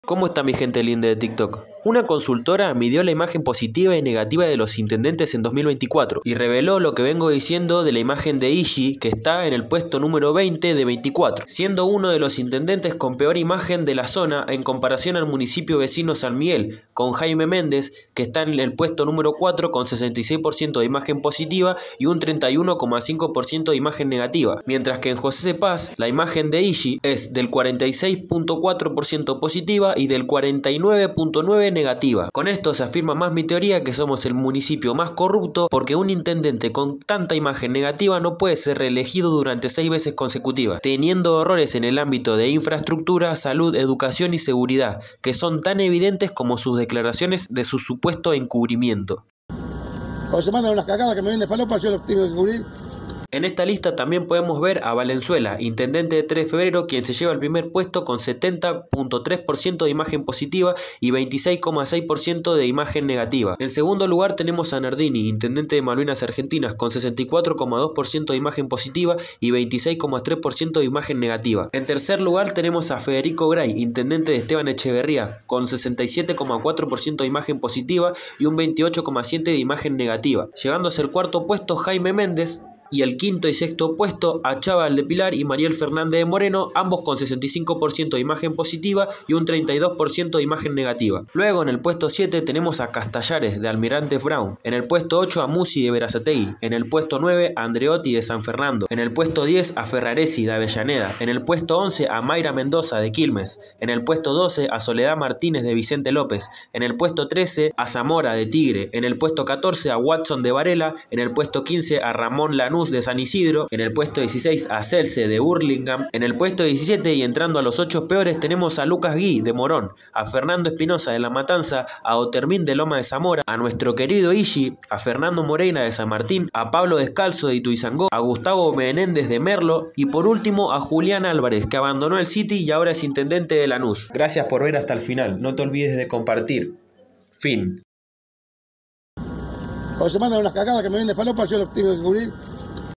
imagen de intendentes de PBA 2024
medición de imagen negativa/positiva de intendentes de PBA y ishii mágicamente tiene 50%de imagen negativa contra 46 positiva...